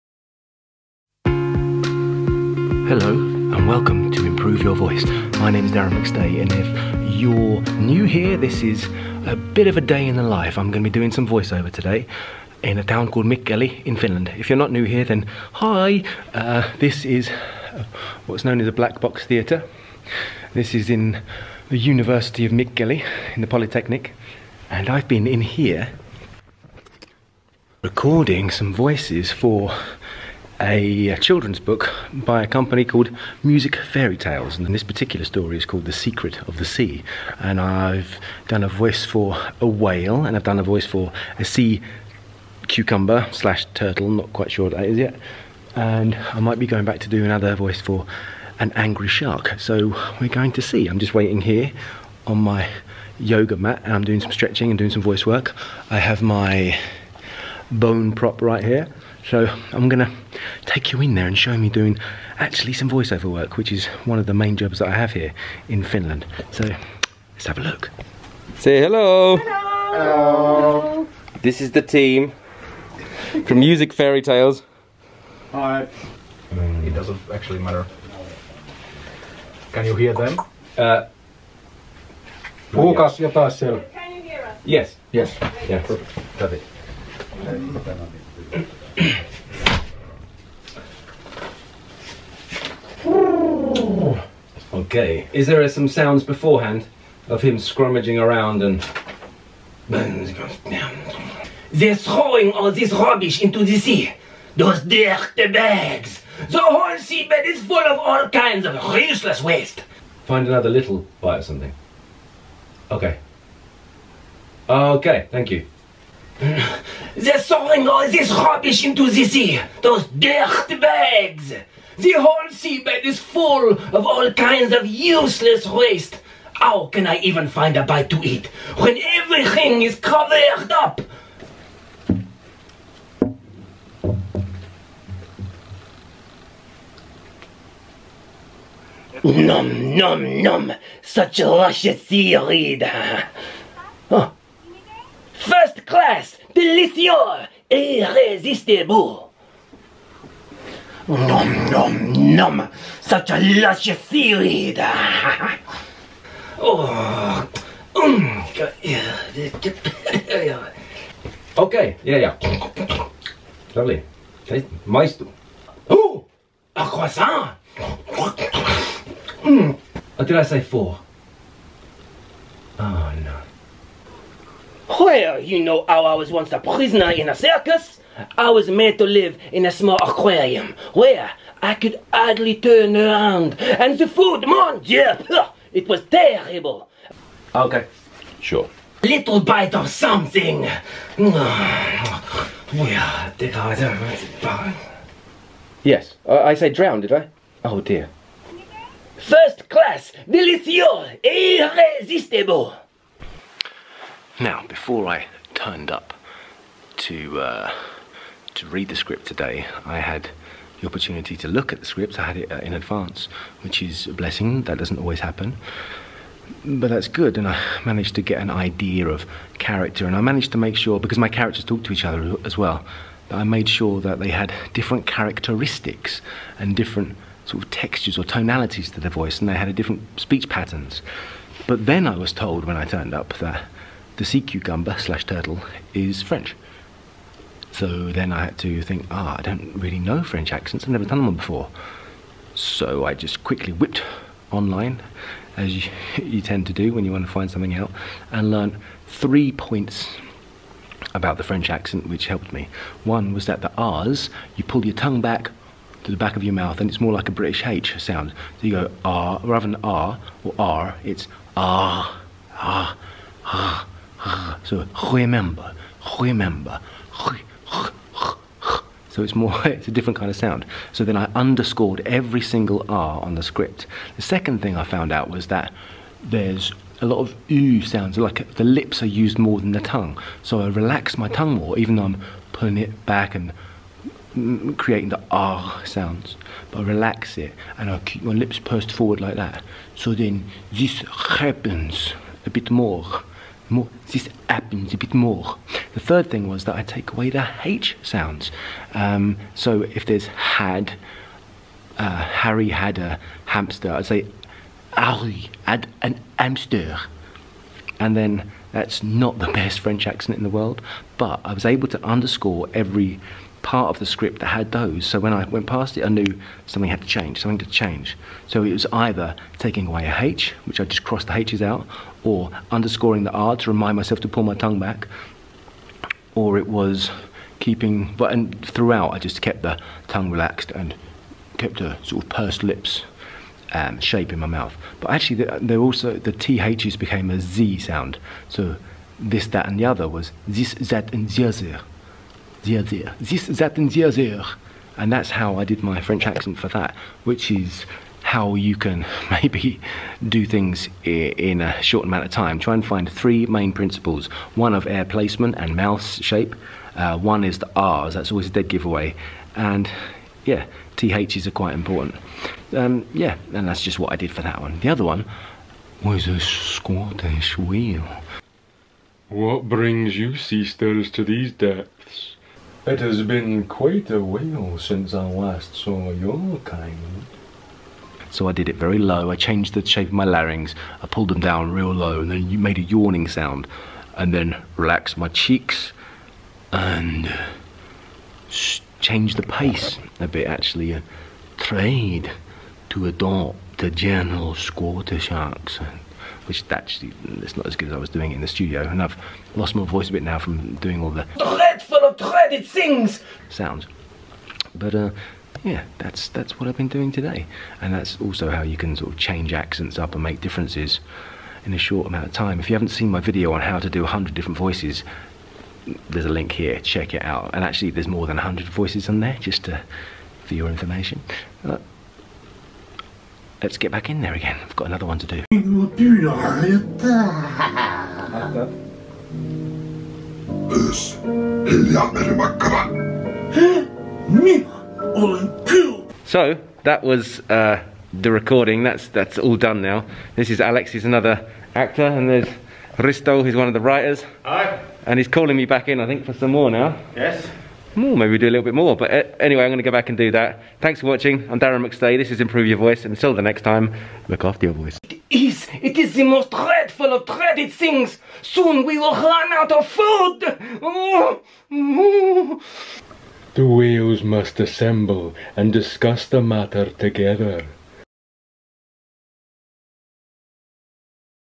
A voiceover artist asked to speak like whale and sea cucumber
In this video Voice Coach Darren McStay takes on the challenge of creating three uniquely different sounding voices (and explains how he did it) for an upcoming childerns story book, created by the wonderful Music Fairytales Oy.

Practice daily with Darren's premier online home training course 'Improve Your Voice' below...